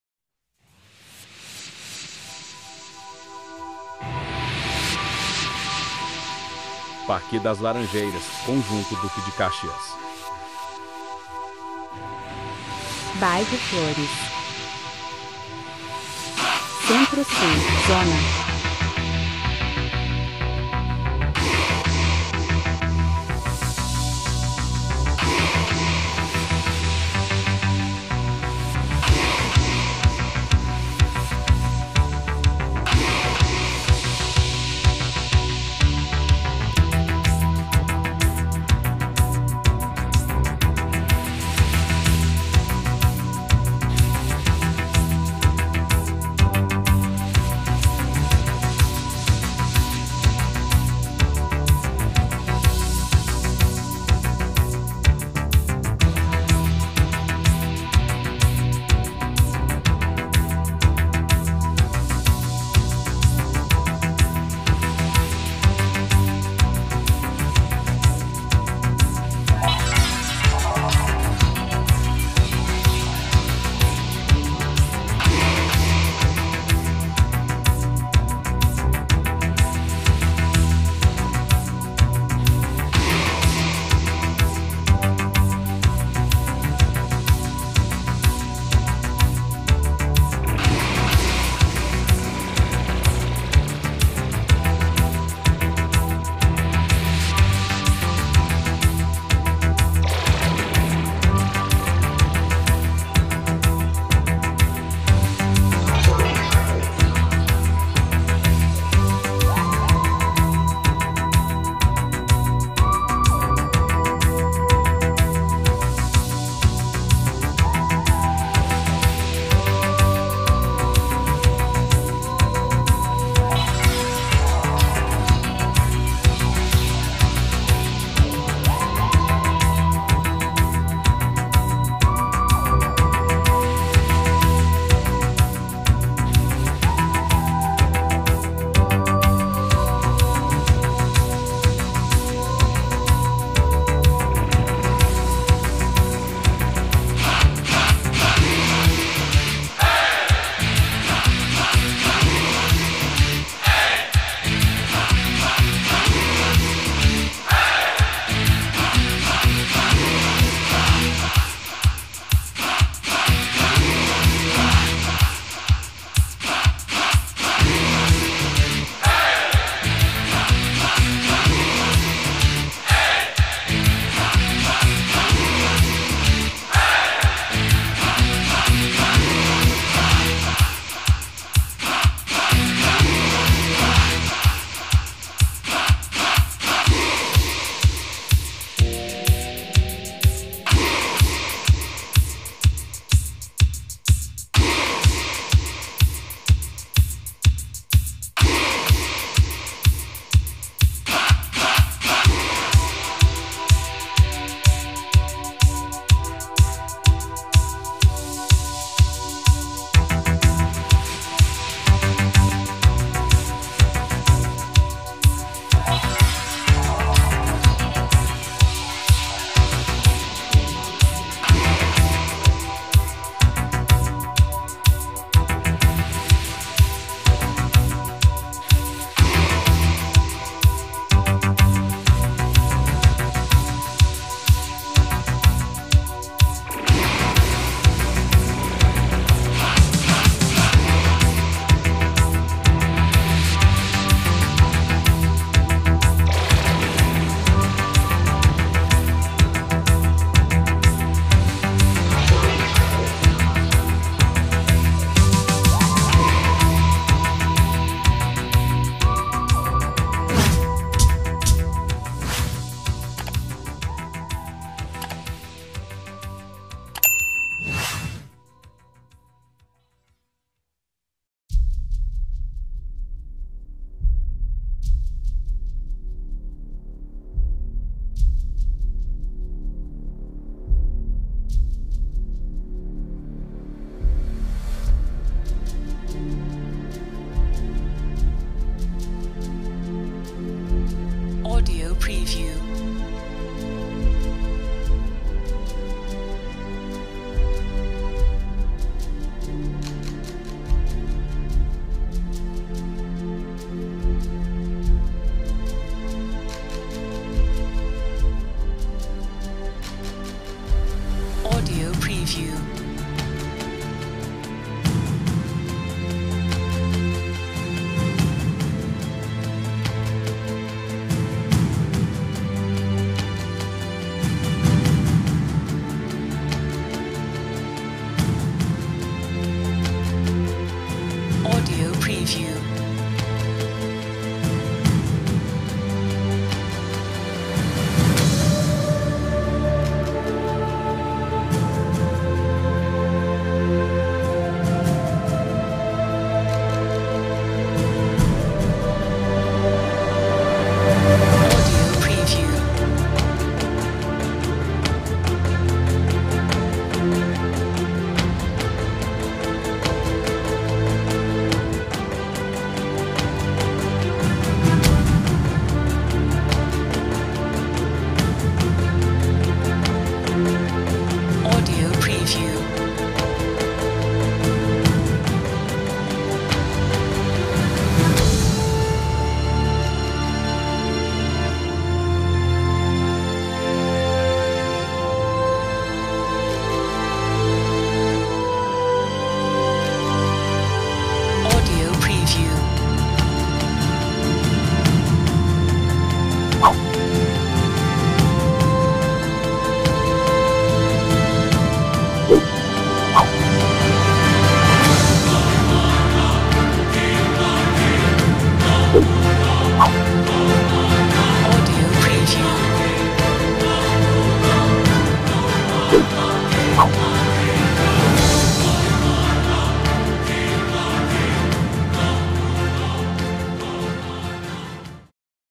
Manaus vista de cima Flores, parque das Laranjeiras, Torquato e Conjunto Duque de Caxias
Video partindo do conjunto duque de caxias no parque das laranjeiras, bairros de flores , mostrando trechos da avenida torquato Tapajos e Senador Raimundo Parente

Contatos
facebook / twitter Santos Drone
Instagram Krieljaby